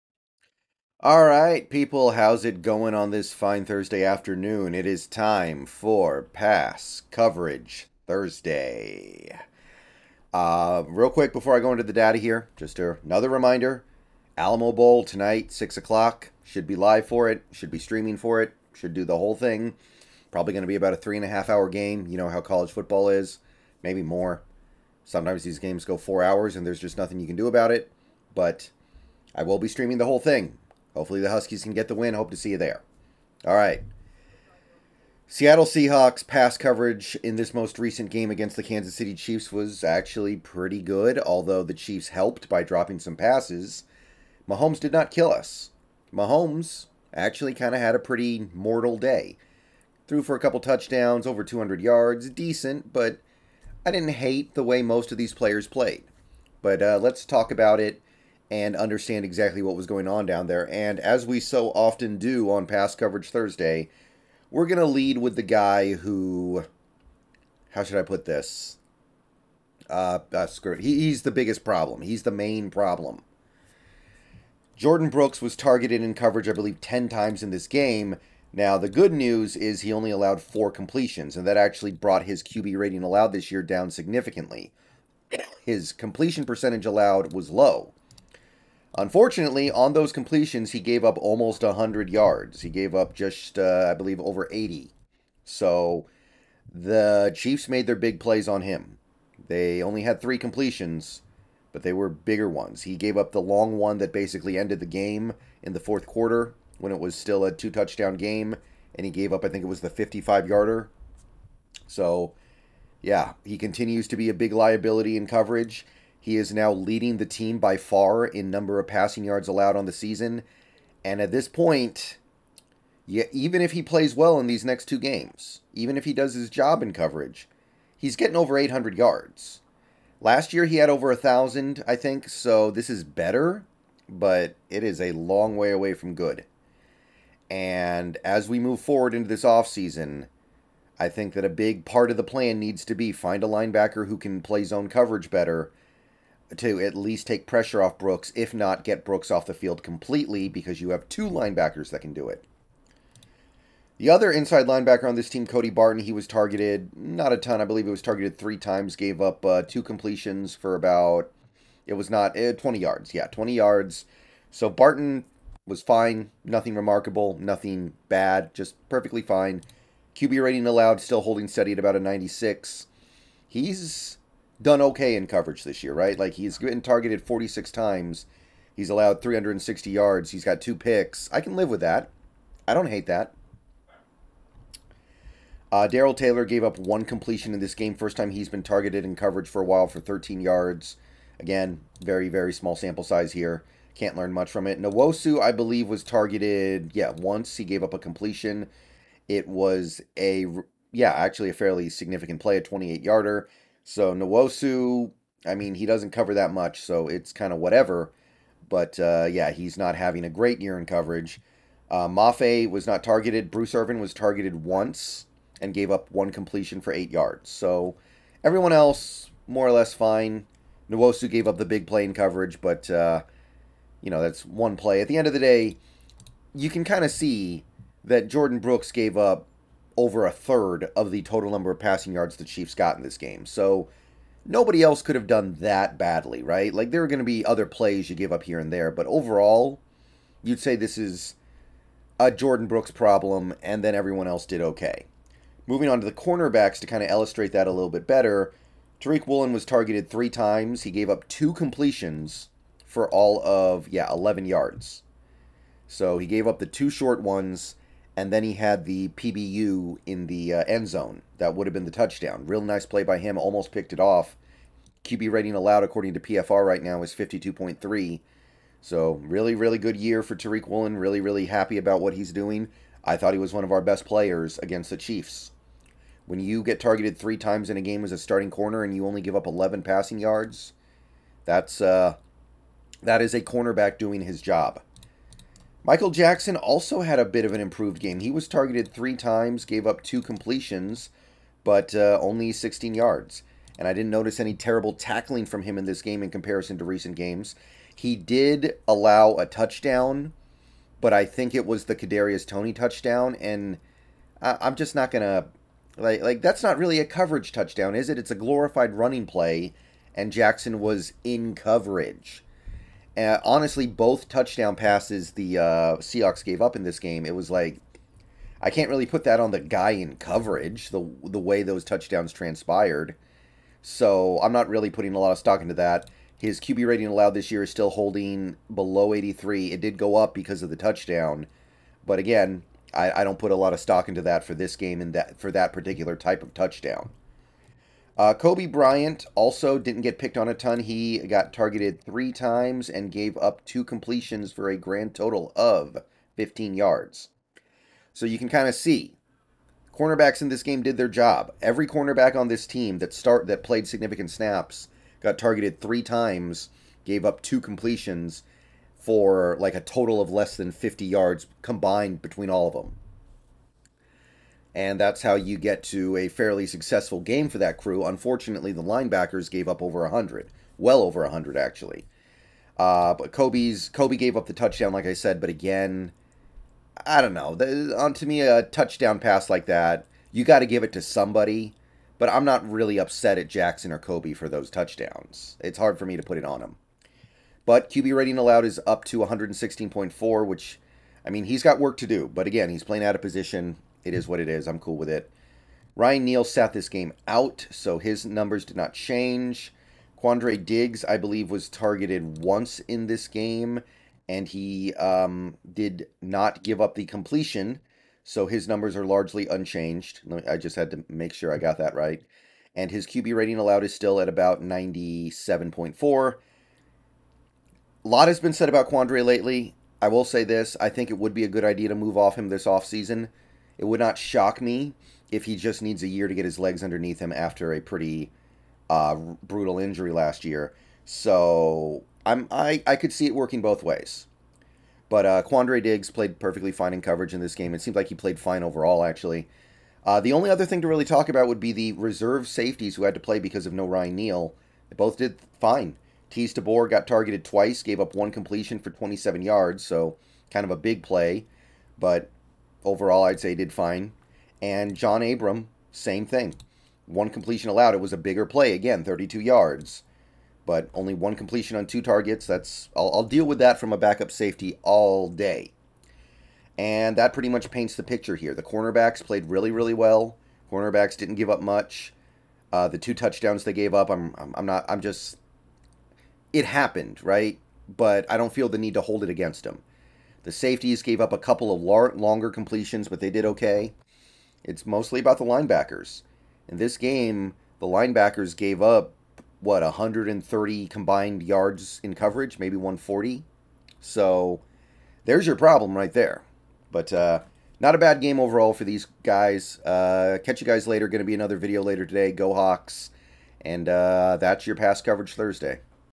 Pass Coverage Thursday: Linebackers get beat up, corners play a great game
Really solid overall with one exception.